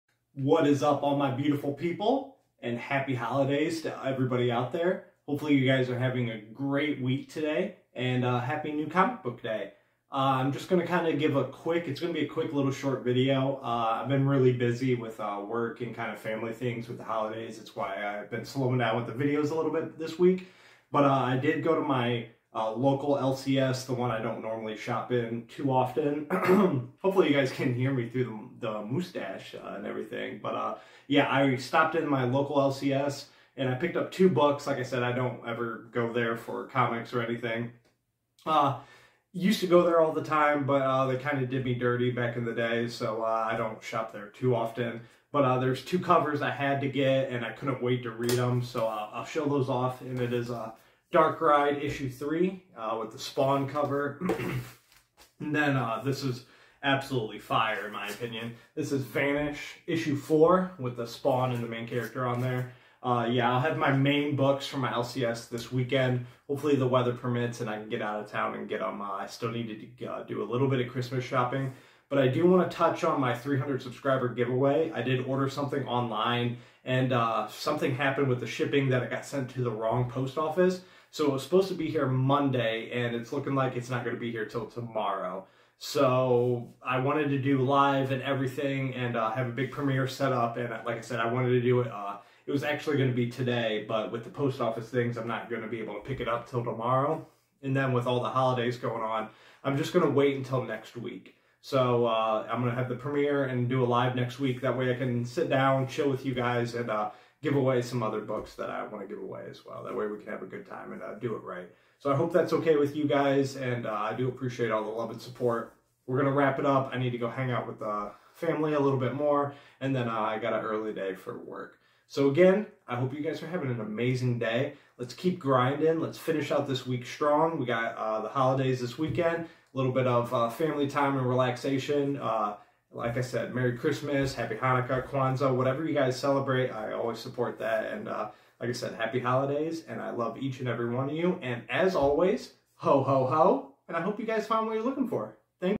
"Tiny" NCBD Haul/ 300 Subscribe Giveaway update.
Hope we are having an amazing week and are having an even better holiday week. 🎄🎄 Today I bring you an update to my 300 subscriber giveaway and also very small NCBD Haul until this weekend lol. Let's all finish out this week strong and head into the new year on the right foot.